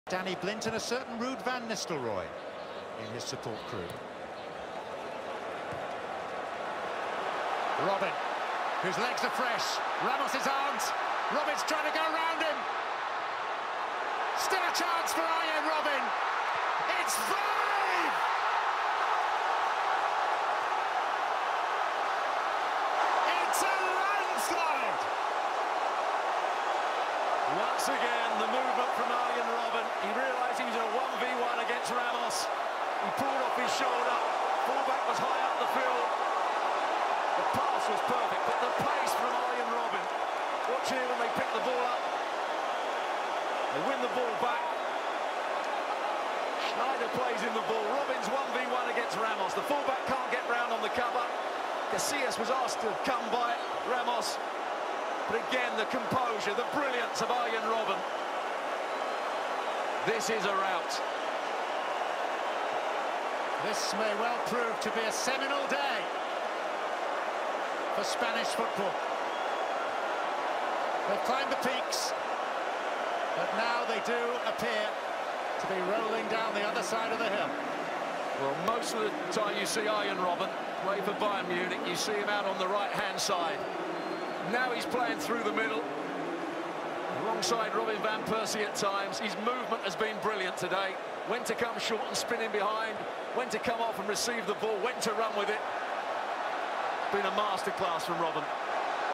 Robben's World Record Speed ● World Cup 2014 ● Fastest Run Ever
Arjen Robben sprint vs Sergio Ramos,  wrapping up the last goal during the Netherlands' domination against Spain in Brasil 2014.

Robben World Cup